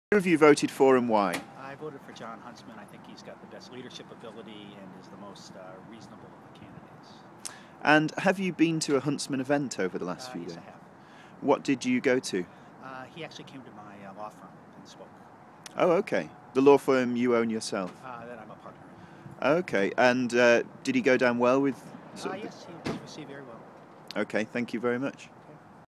New Hampshire primary voter David Barnes -- the Guardian
Manchester man David Barnes on who he voted for and why